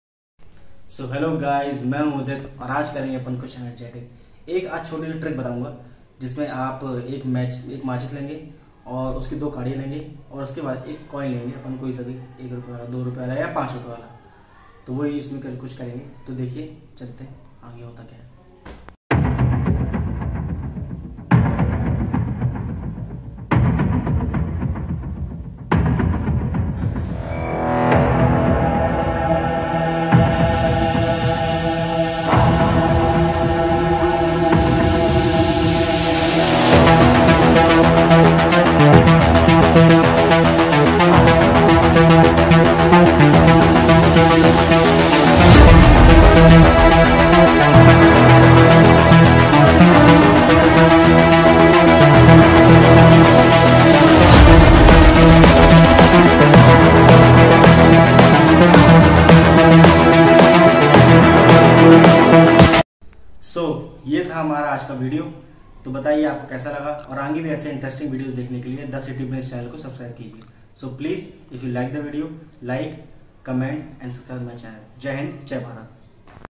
हवा में उड़ने वाली माचिस देखिए | TRICK USING MATCHSTICKS 😜 | CITY PRINCE
माचिस का कमाल देखिए | TRICK USING MATCHSTICKS 😜 | CITY PRINCE,
Thanks to Caleb M. Rosenberg for this puzzle which he learned from a waiter named Bomin for the Philippines. Move 1 matchstick to turn the donkey.